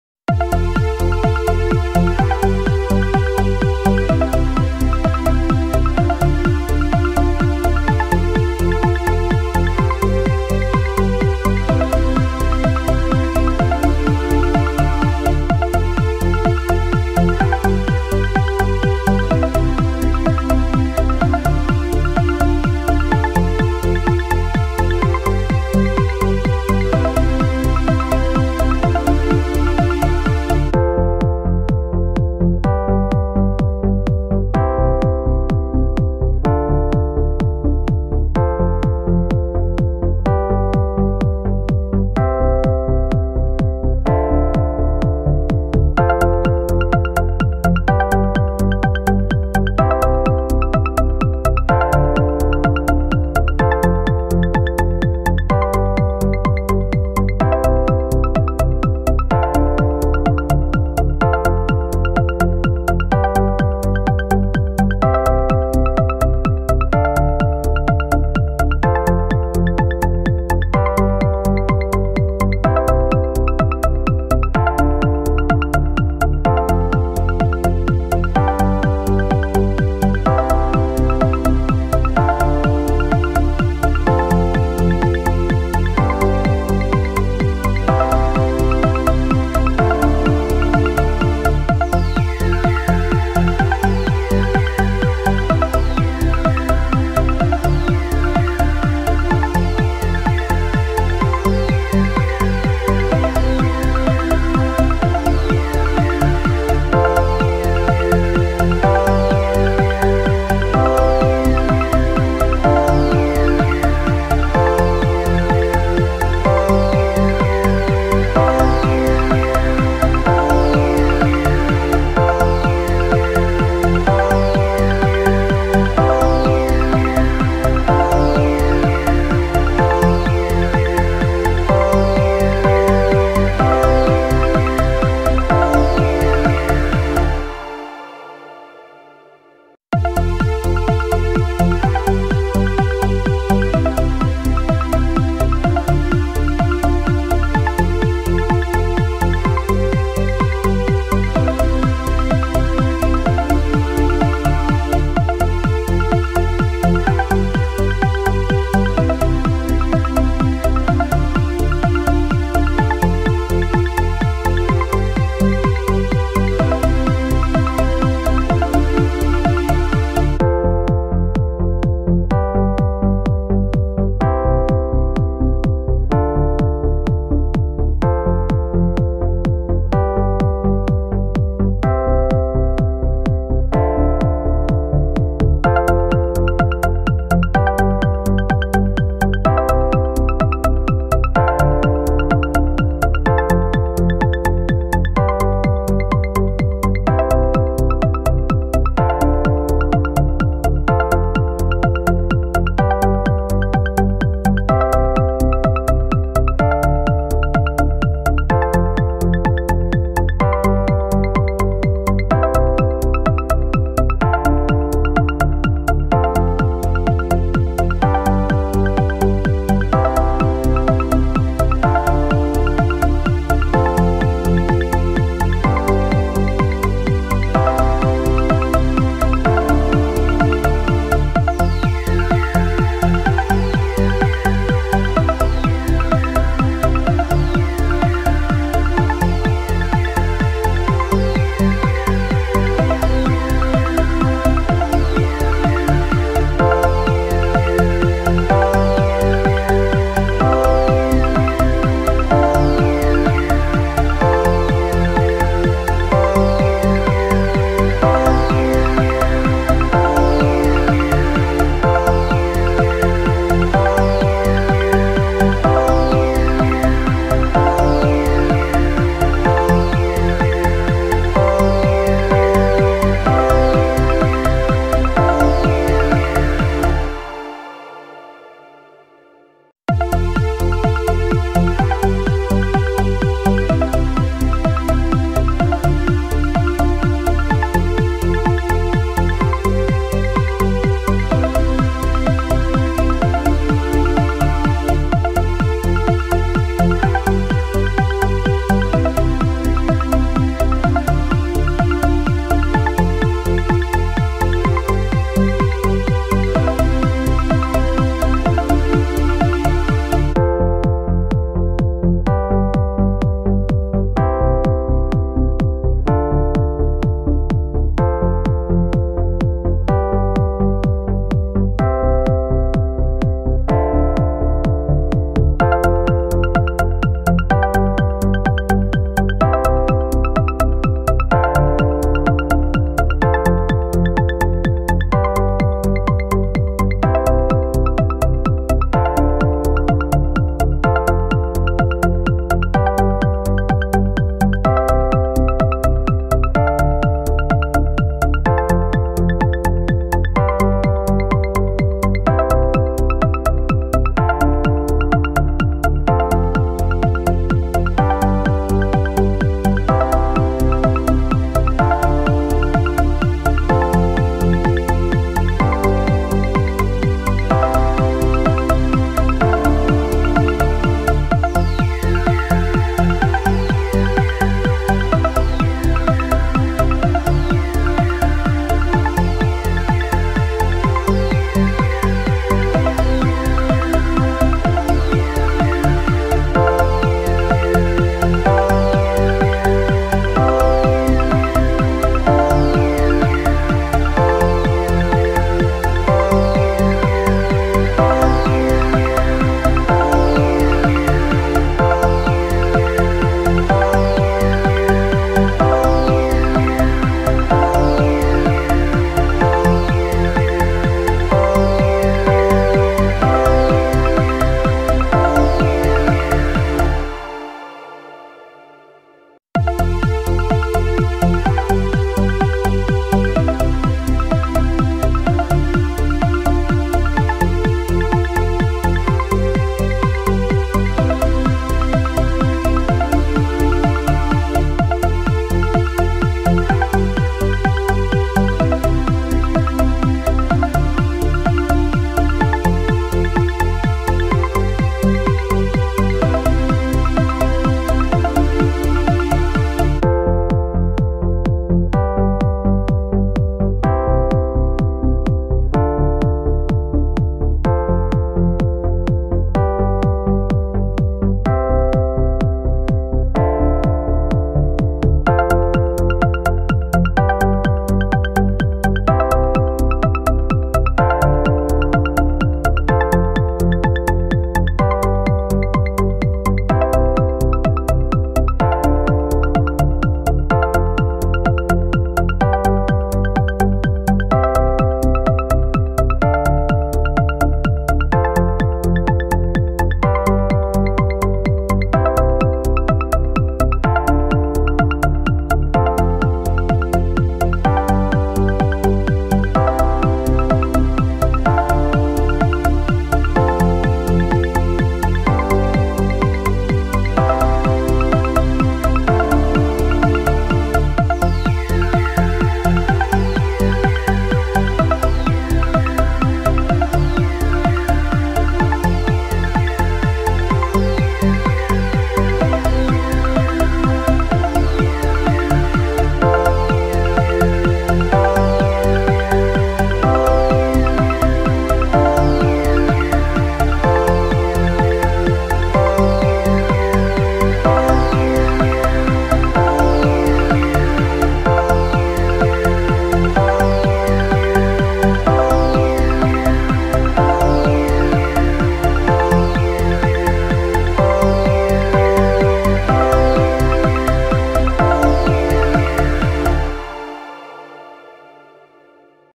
In The Zone Soft Instrumental Electronic Music For Deep Focus
This Soft Instrumental Electronic Music is perfect for Deep Focus whenever you need to get lost in deep concentration